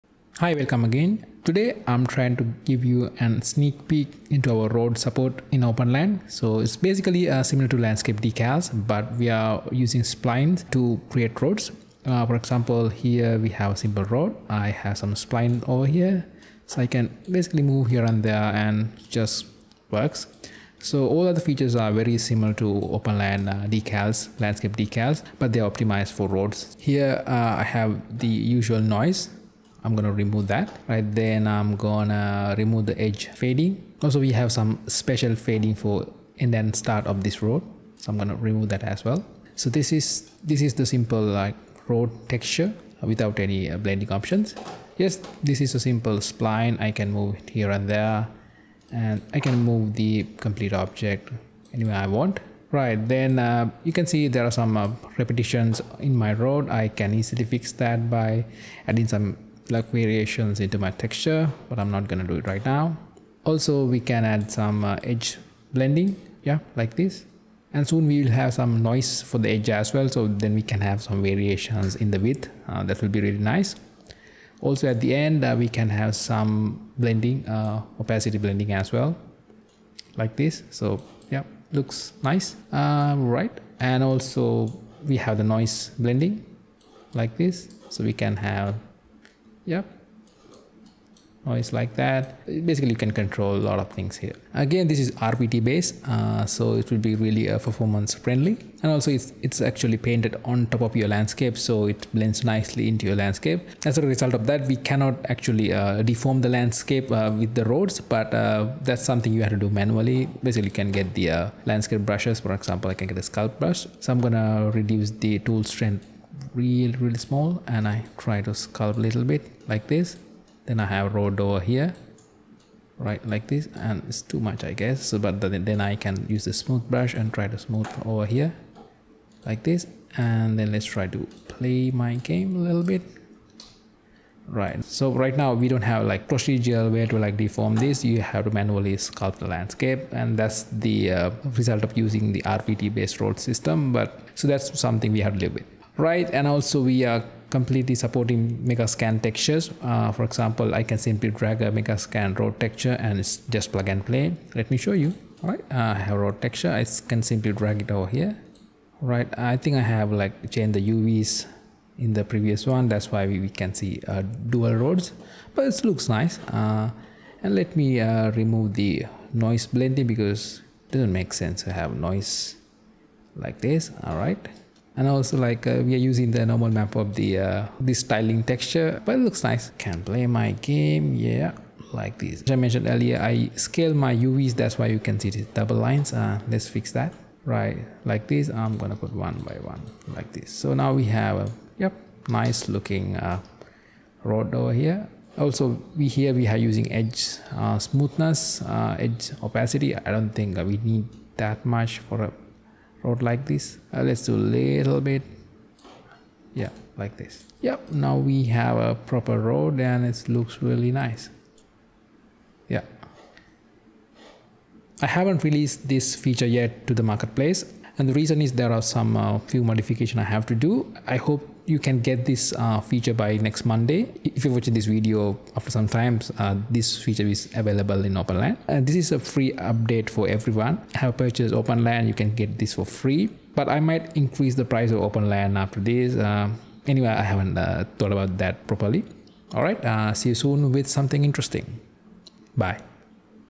Sneak Peek: OpenLand Roads for Unreal Engine (RVT based road system)
This is an RVT based road system for Unreal Engine. It's plug & play & based on splines. So, you have complete control & blend nicely with the landscape.